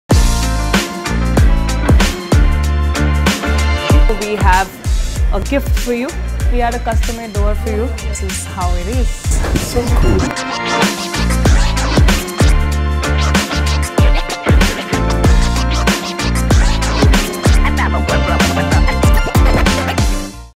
Boutique Living
Boutique Living gifted the celebrity chef Ranveer Brar  with a customised dohar at Pinkvilla Style Icons Award 2022, as a token of appreciation for his participation in our cause Karenge Poore Sapne Adhoore.